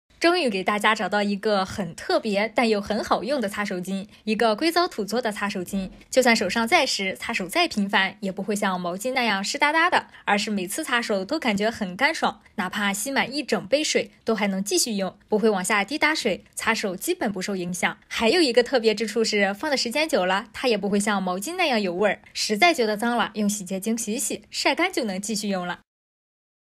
This diatomaceous earth towel is quick drying after wiping your hands, and it is still dry after sev
This diatomaceous earth towel is quick-drying after wiping your hands, and it is still dry after several times of wiping, not as wet as a towel